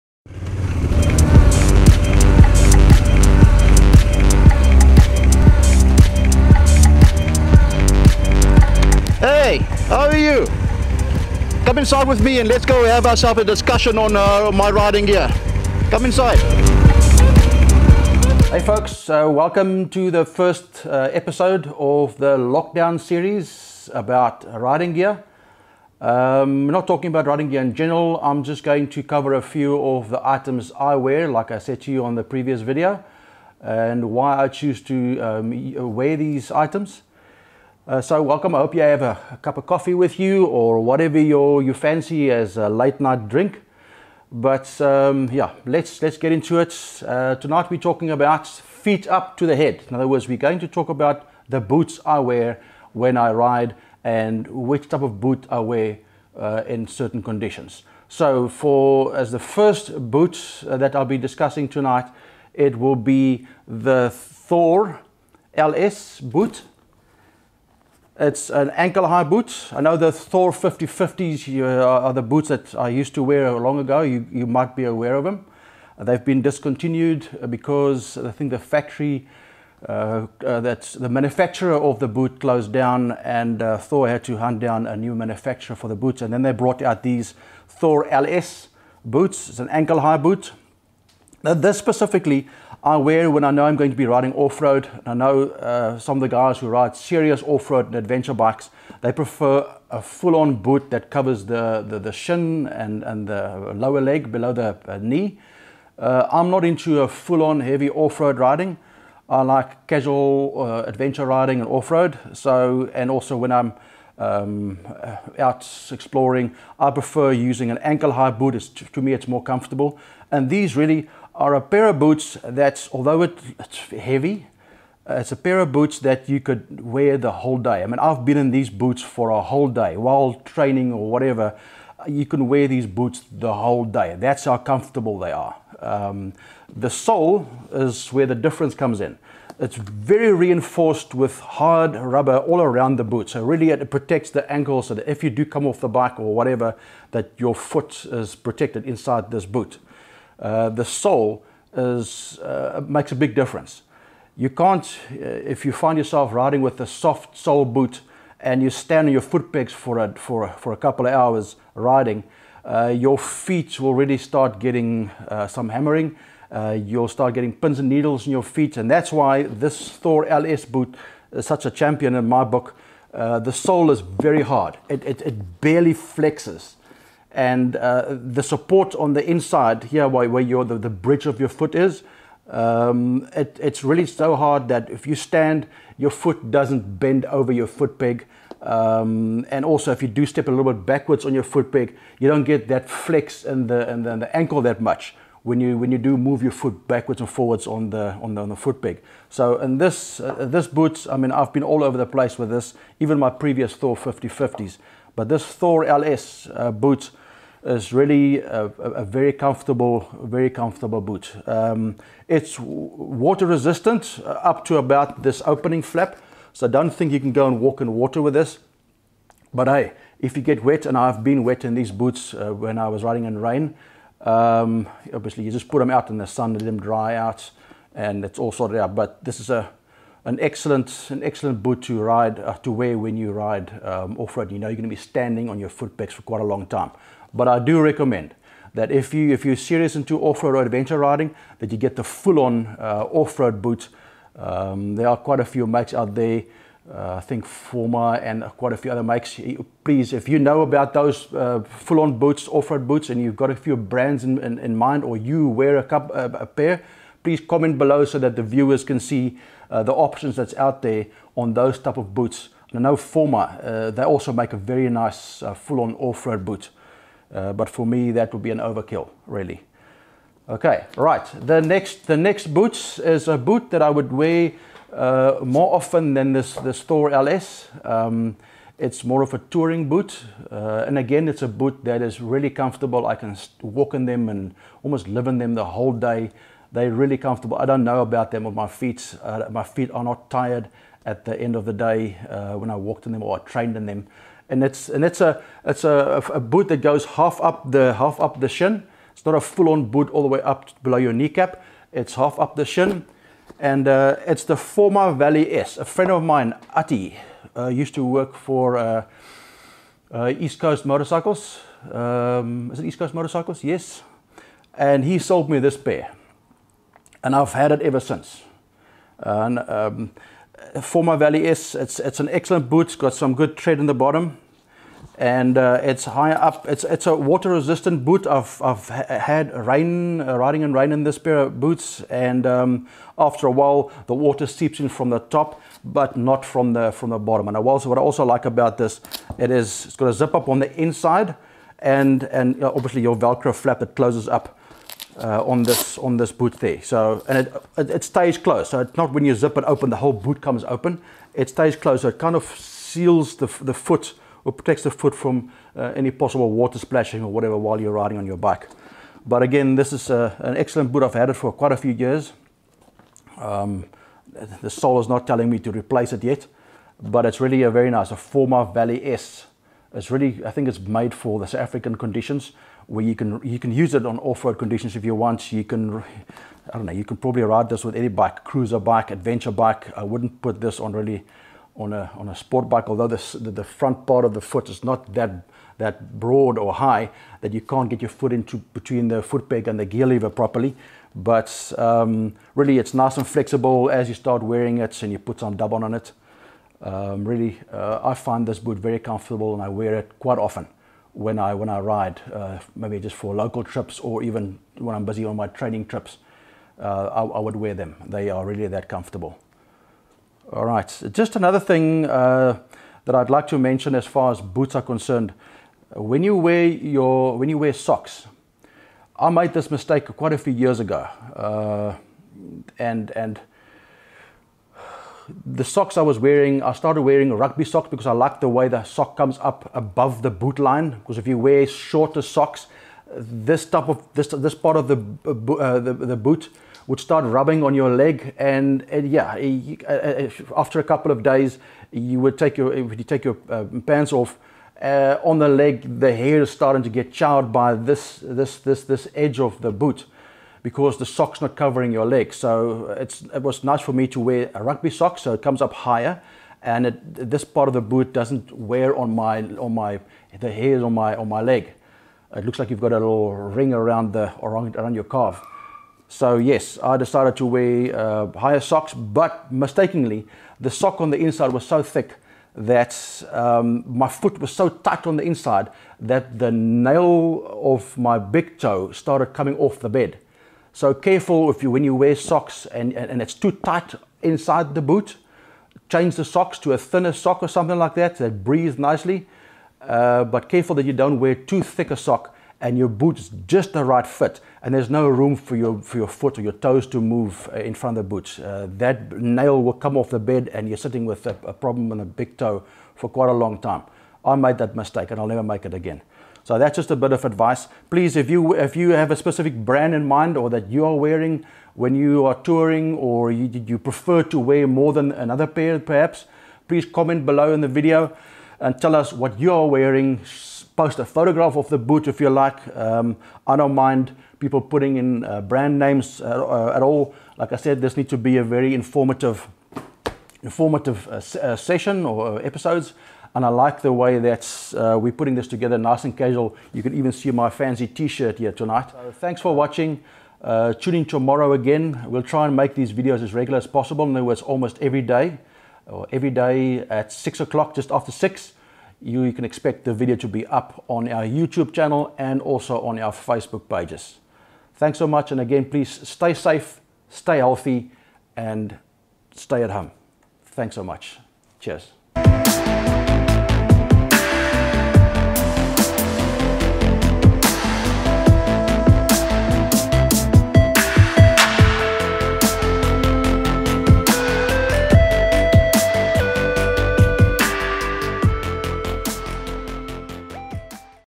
Lockdown Episode 1 - My Motorcycle Boots
Hein talks about his motorcycle boots, the Forma Valley S Touring Boot and the Thor LS Adventure Boot.